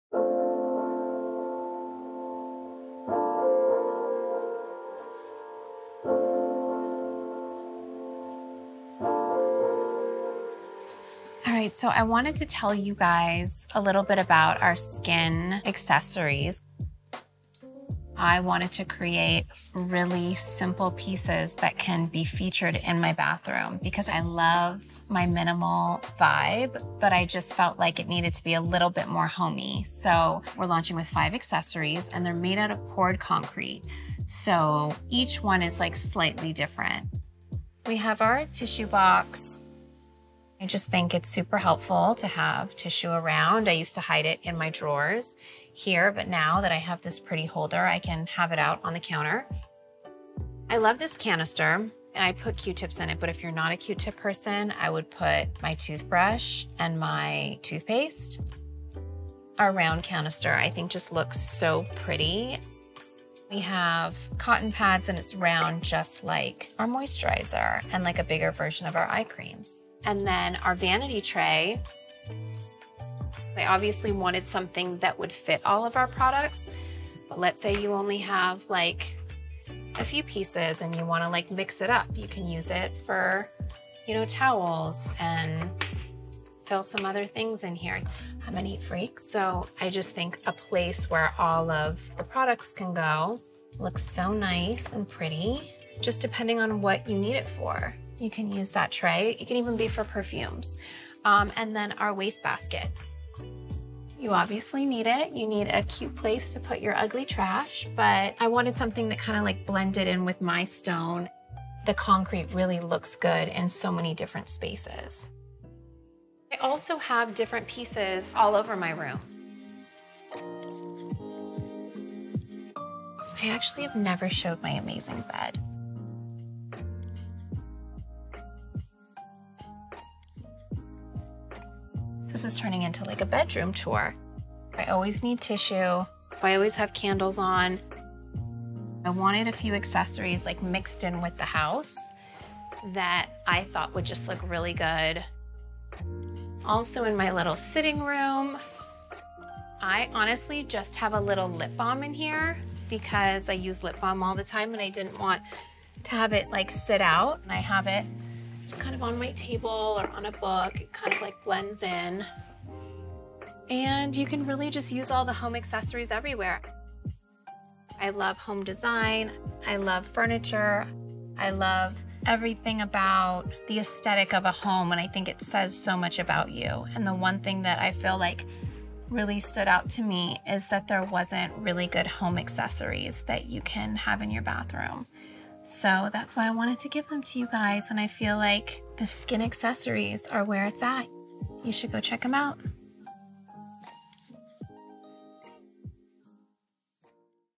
SKKN BY KIM Home Accessories are here!
Designed to complement the skincare line, the Home Accessories collection marries art and functionality for a space that feels modern and elevated.